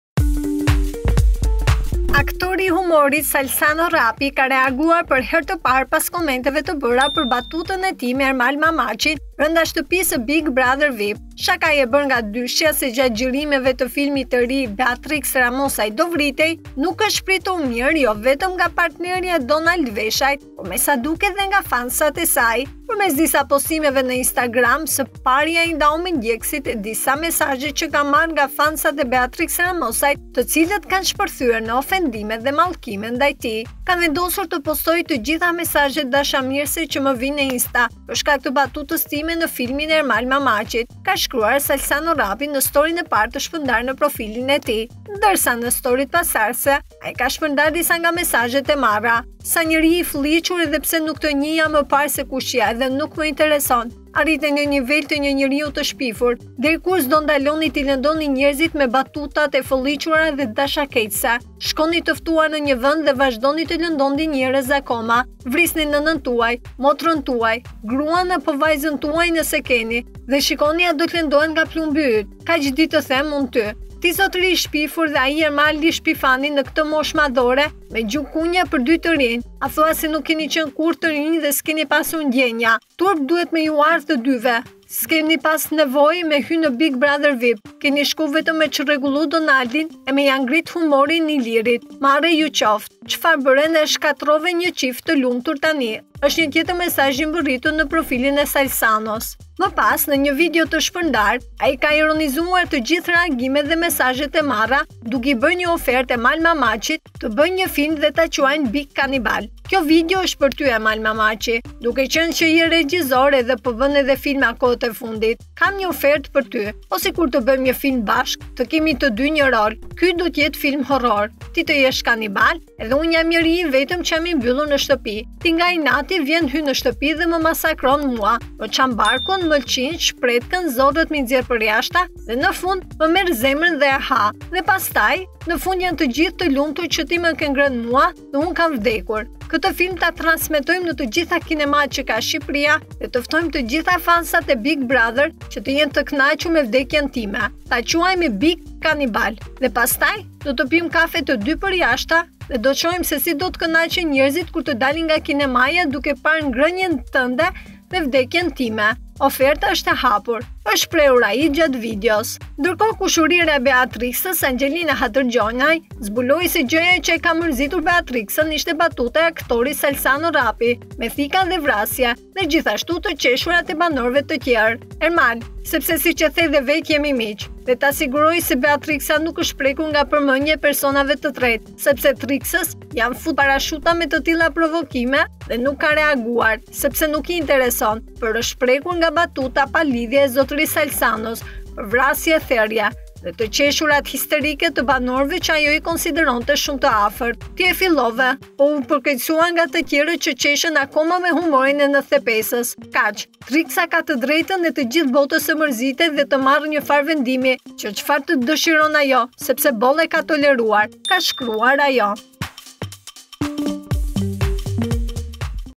“O i fëlliqur!” “Na shkatërrove një çift”- Mori mallkime dhe ofendime nga fansat e Beatrix Ramosajt.
“O i fëlliqur!” “Na shkatërrove një çift”- Mori mallkime dhe ofendime nga fansat e Beatrix Ramosajt, reagon për herë të parë Salsano Rrapi

Salsano Rrapi ka reaguar për herë të parë pas komenteve të bëra për batutën e tij me Ermal Mamaqin brenda shtëpisë së Big Brother VIP. Shakaja e bërë nga dyshja se gjatë xhirimeve të filmit të ri “Beatrix Ramosaj do vritej”, nuk është pritur mirë jo vetëm nga partnerja e Donald Veshajt, por me sa duket edhe nga fansat e saj.Përmes disa postimeve në Instagram, së pari ai ndau me ndjekësit disa mesazhe që ka marrë nga fansat e Beatrix Ramosajt, të cilët kanë shpërthyer në ofendime dhe mallkime ndaj tij.
“Kam vdenosur të postoj të gjitha mesazhet “dashamirëse” që më vijnë në insta, për shkak të batutës time në filmin e Ermal Mamaqit.”, shkruan Rrapi në storyn e parë të shpërndarë.

Ndërsa në storyt pasardhëse ai ka shpërndarë disa nga mesazhet e marra.

“Sa njeri i felliqur edhe pse nuk te njihja pare se kush je dhe s’me intereson arrite ne nje nivel te nje njeriu te shpifur, deri kur s’do ndaloni ti lendoni nierzit me batutat e felliqura e dashakegese, shkoni te ftuar ne nie vend dhe vazhdoni te lendoni njerez akoma!!! Vrisni nenen tuaj, motren tuaj, gruan apo vajzen tuaj nese “keni” he shiko a do lendohen nga plumbi yt kaq di te them ty !”
"Ti zotri i shpifur edhe aj Ermal shpifani ne ket mosh madhore me gju kunja per dy te rinje, the se nuk jeni kan kurr te rinje ju e skeni pas ndjenja, turp duhet me ju ardh dyve skeni pas nevoj as me shku bilemi po jeni hi ne bbv me qellim mi fol ato fjal per me qrregullu Donaldin e me grit Ilirin marre ju koft”.
"Cfarë bere na e shkaterrove nje cift të lumtur tani".
Më pas në një video të shpërndarë ai ka ironizuar të gjithë reagimet dhe mesazhet e marra, duke i bërë një ofertë Ermal Mamaqit të bëjnë një film dhe ta quajnë “Big Kanibal”.
“Kjo video është për ty Ermal Mamaqi. Duke qenë se je regjisor edhe po bën edhe filma kohët e fundit. Kam një oferë për ty: Po sikur të bëjmë bashkë një film, të kemi të dy një rol. Ky filmi të jetë horror. Ti të jesh kanibal edhe unë jam një njeri që jam i mbyllur në shtëpi. Ti nga inati vjen hyn në shtëpi dhe mër masakron mua, më çan barkun, mëlçinë, shpretkën, zorrët, mi nxjerr përjashta dhe në fund më merr zemrën edhe e ha.
Dhe pastaj në fund janë të gjithë të lumtur që ti më ke ngrënë mua dhe unë kam vdekur. Këtë film ta transmetojmë në të gjitha kinematë që ka Shqipëria dhe të ftojmë të gjithë fansat e Big Brother, do jenë të kënaqur besoj të gjithë me vdekjen time. Ta qujamë “Big Kanibal”. Dhe pastaj do të pimë kafe të dy përjasha dhe do të shofim se si do kënaqen njerëzit kur tët dalin nga kinemaja duke parë ngrënien tënde dhe vdekjen time. Oferta është e hapur.”, u shpreh ai gjatë videos.
Kushërira e Beatrix, Anxhelina Hadërgjonaj zbuloi se gjëja që e ka mërzitur Beatrixën ishte ‘batuta e aktorit, Salsano Rrapi, me thika dhe vrasje dhe gjithashtu të qeshurat e banorëve të tjerë’.
“Ermal sepse siç e the dhe vet jemi miq, dhe ta siguroj nuk eshte prek nga permendja e personave te tret sepse Trixes i jan fut parashuta me te tilla provokime dhe nuk ka reaguar, sepse nuk i intereson) Por eshte prekur nga batuta palidhje e Zotri Salsanos(per vrasje e therje) dhe te qeshurat histerike te banoreve qe ajo i konsideronte shume te afert. Ti e fillove po u perkeqesua nga te tjeret qe qeshin akoma me humor te 95′. Kaq! Trixa ka te drejten e te gjith botes te merzitet e te mari fare vendimi te doje sepse boll ka toleruar”, ka shkruar ajo.